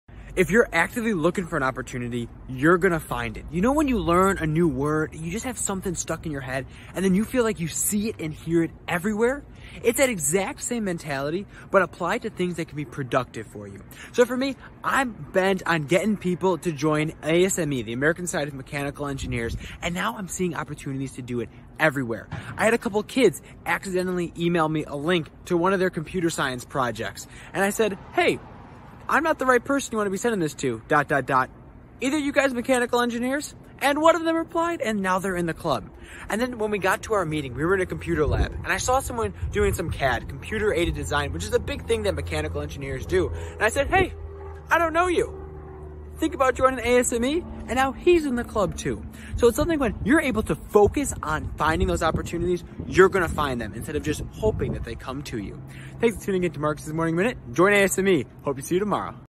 Ever learn something new and realize it shows up everywhere? - Day 297
That's the power of focusing on opportunities 👀

I'm pretty focused on recruiting people to join ASME Purdue (you can probably tell by how I mention it almost every day lol)

So when even an inkling of an opportunity arises

I'm ready to hop on it 🐇

It's just like if you start thinking of a yellow car, you'll start seeing them everywhere!

What will you start focusing on to best utilize your opportunities❓